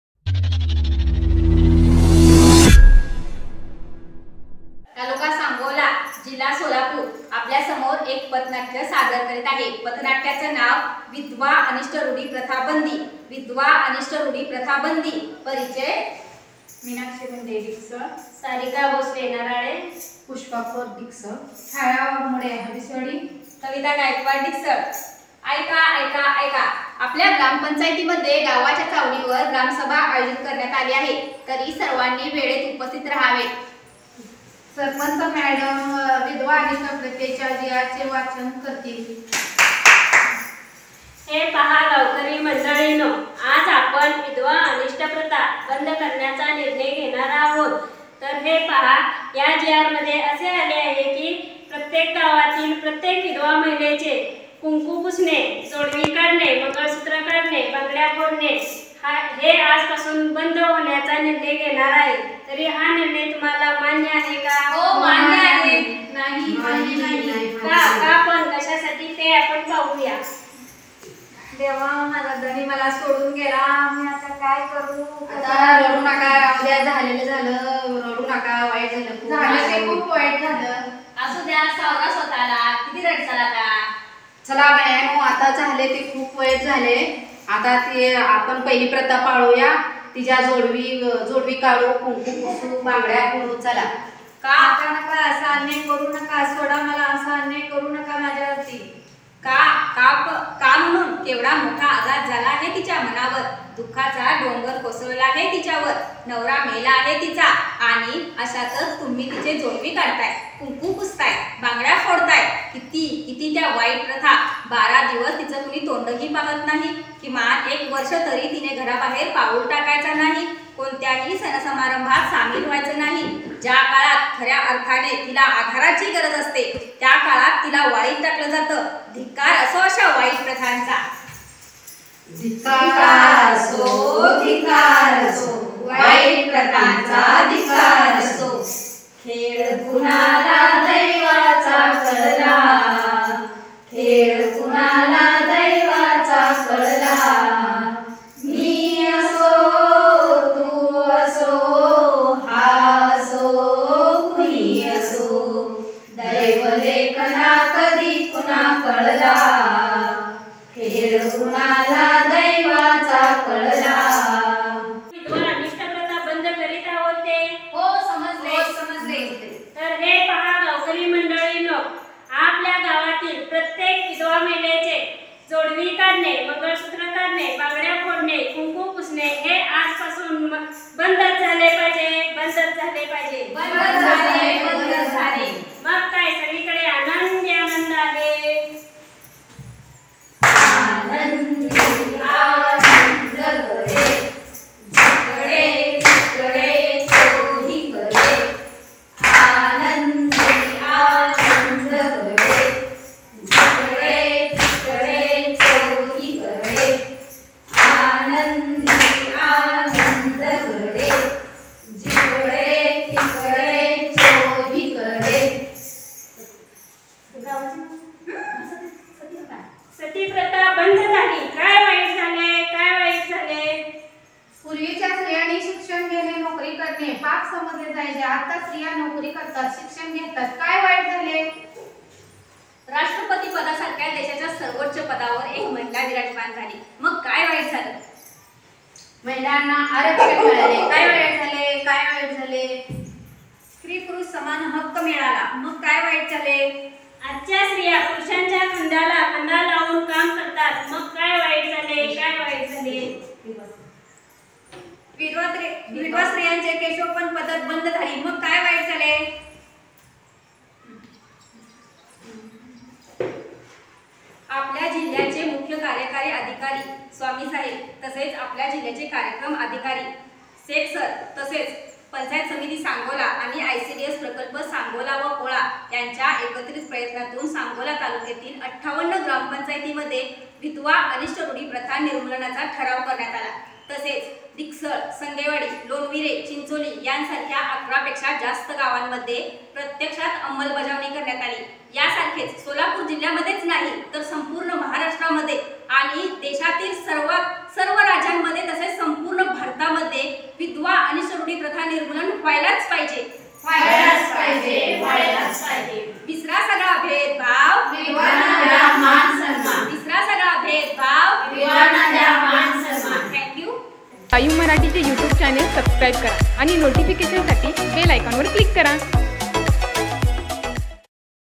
डिकसळ येथे ‘विधवा प्रथा बंद झाली पाहिजे’ पथनाट्याचे सादरीकरण..!
सांगोला तालुक्यातील डिकसळ येथे ‘विधवा प्रथा बंद झाली पाहिजे’ या विषयावर अंगणवाडी सेविका मीनाक्षी भंडे आणि त्यांच्या सेविकांनी पथनाट्याद्वारे उत्कृष्ट सादरीकरण केले.